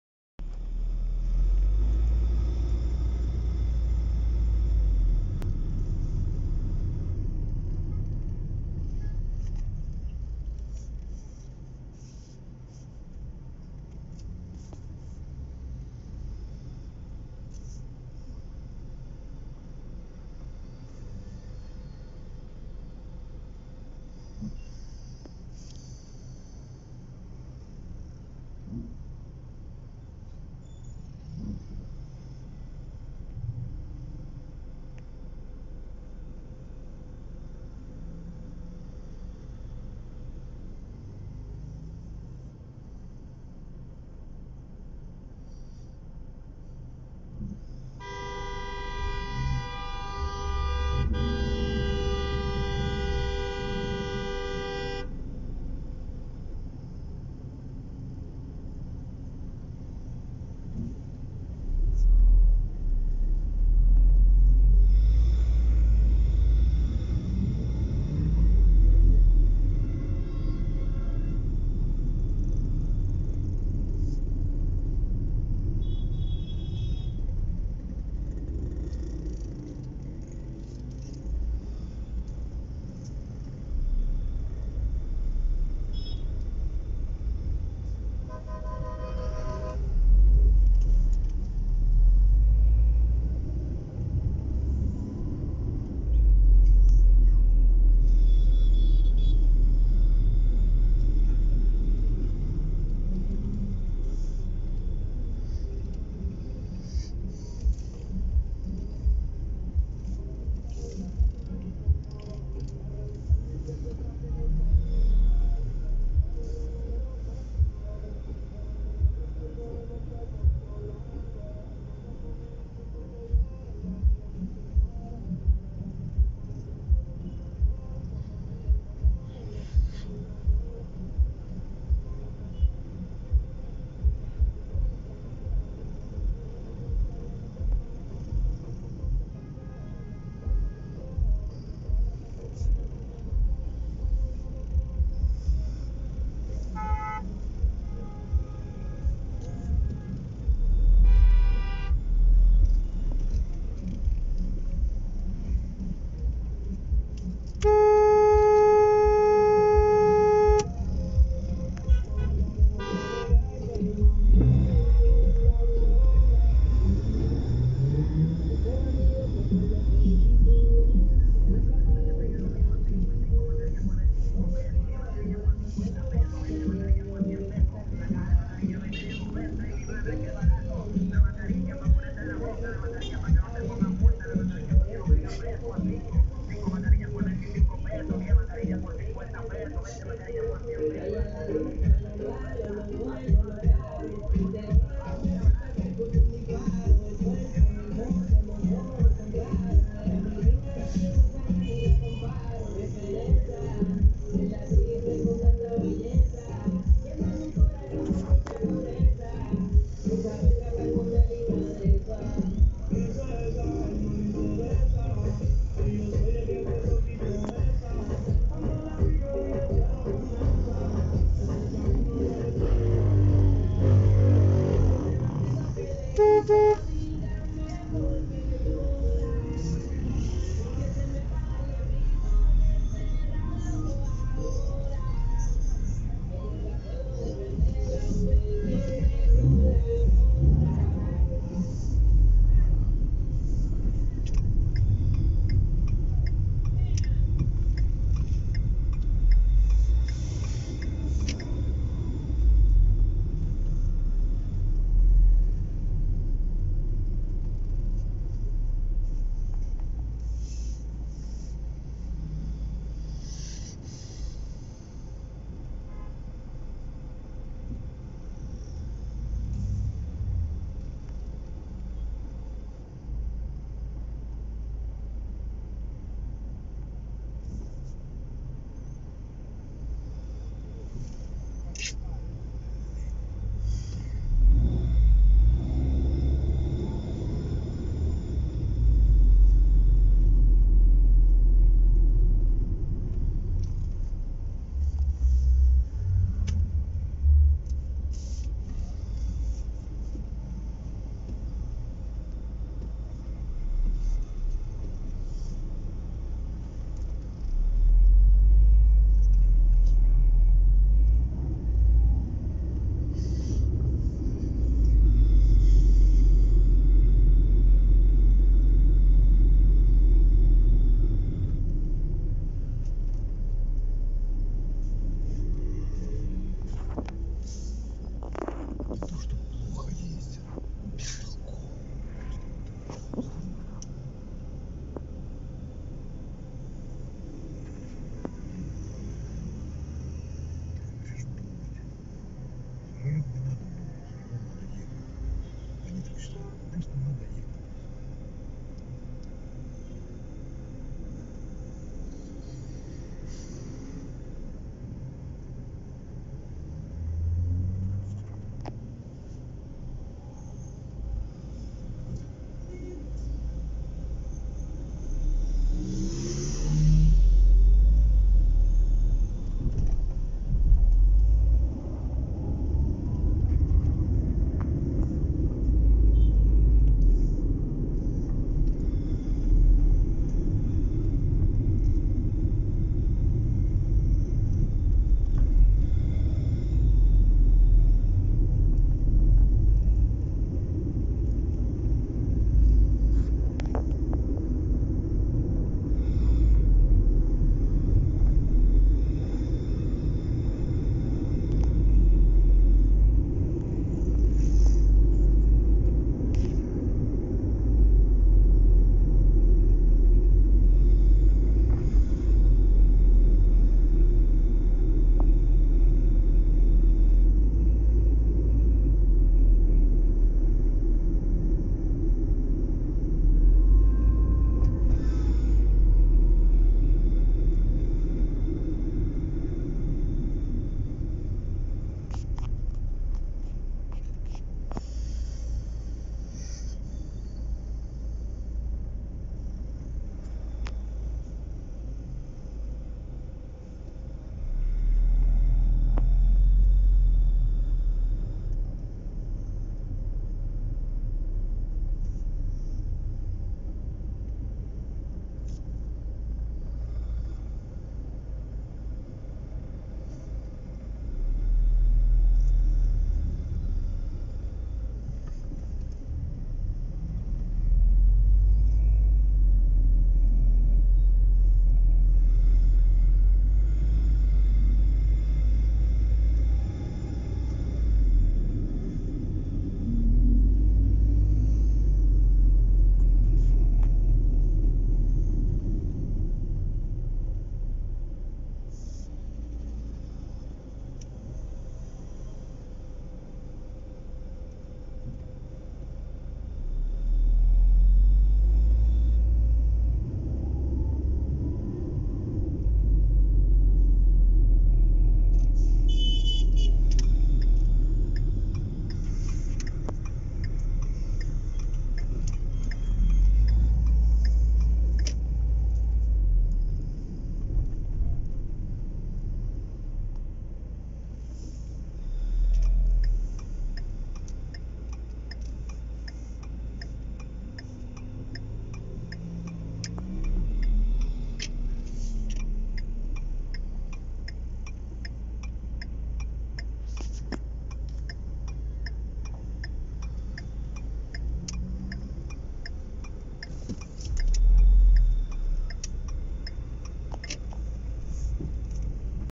Хаотичное движение на улицах Сан Педро де Макорис, 4 февраля 2021 г.
Вождение машины в Доминикане - это очень большой стресс. Очень многие доминиканские водители нарушают пдд. А мотоциклисты так вообще не знаю что существуют какие-то правила. Они выезжают на встречку, обгоняют с любой стороны, очень часто сшибают машинам боковые зеркала. За несколько минут до того, как я стала снимать нашу поездку по Сан Педро на видео, прямо на нас на полной скорости выскочил молодой парень на скутере. Он поворачивал налево на одностороннюю улицу, то есть против потока, смотря при этом направо. Еле успел затормозить. Благо, Коля всегда наготове и быстро среагировал.